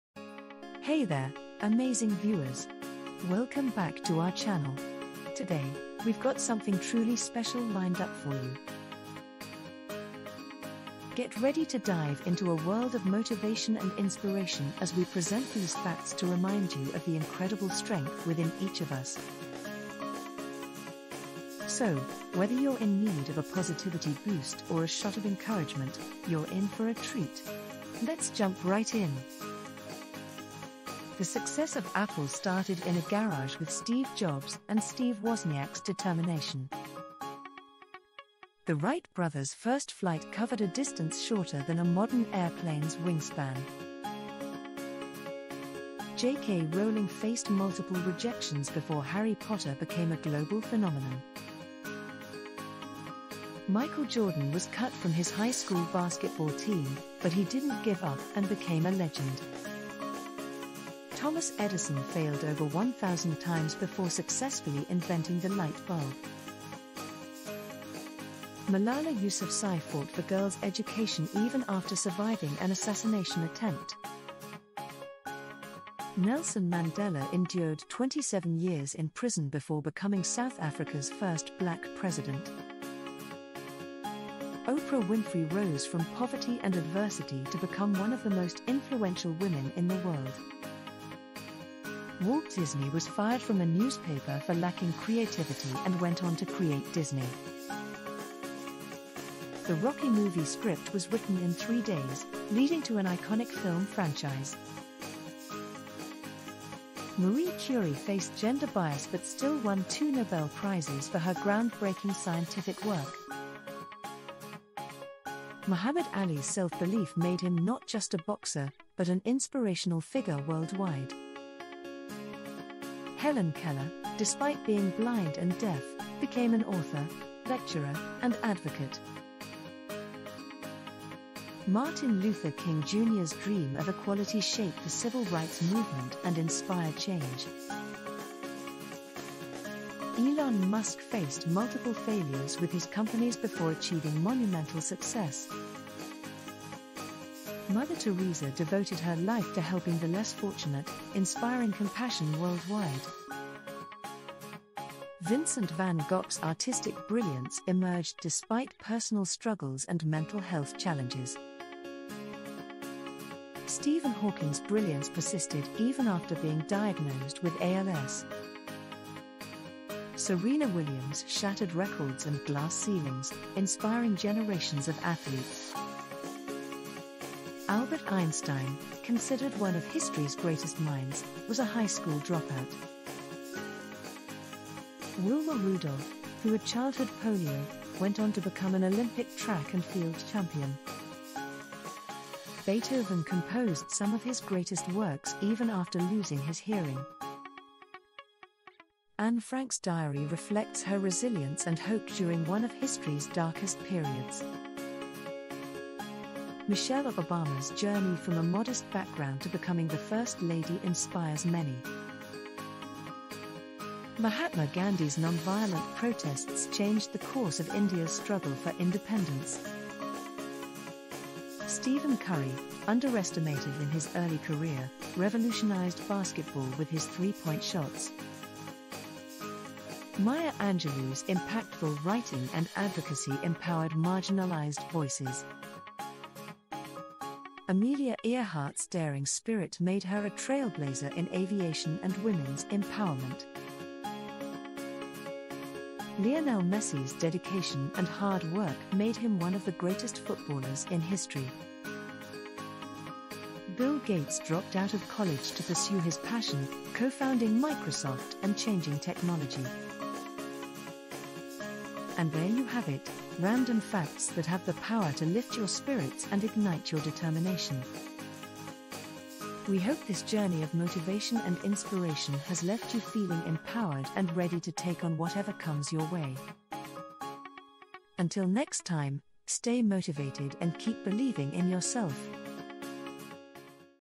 30 Random Facts That Are Motivating and Inspiring
Embark on a journey of motivation and inspiration with "30 Random Facts That Are Motivating and Inspiring." Discover a collection of intriguing facts that showcase the incredible resilience of the human spirit. From stories of triumph over adversity to examples of unwavering determination, these facts will leave you uplifted and ready to tackle challenges head-on. Whether you're seeking a dose of positivity or a reminder of your own potential, this video is a powerful reminder that greatness lies within us all. Join us and be inspired to embrace your dreams, overcome obstacles, and make a positive impact on your life and the lives of others.